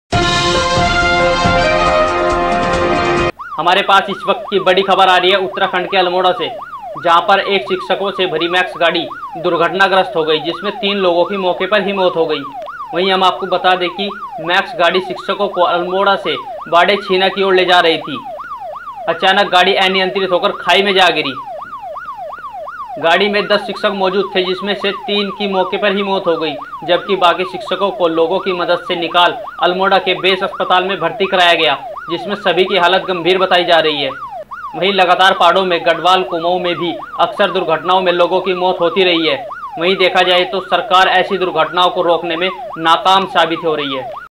शिक्षकों से भरी मैक्स गाड़ी दुर्घटनाग्रस्त
तीन की मोके पर ही मौत